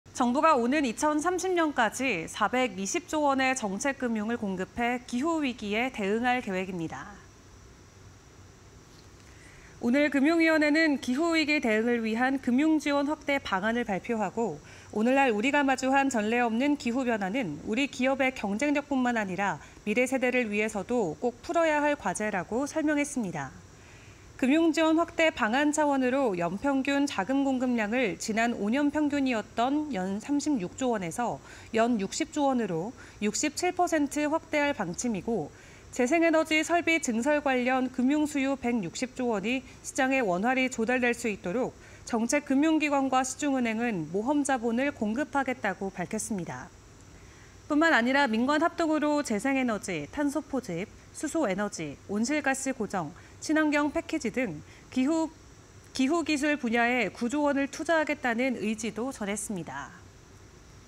정부, 2030년까지 정책금융 420조 투입…기후위기 대응 (20240319)
정부, 2030년까지 정책금융 420조 투입…기후위기 대응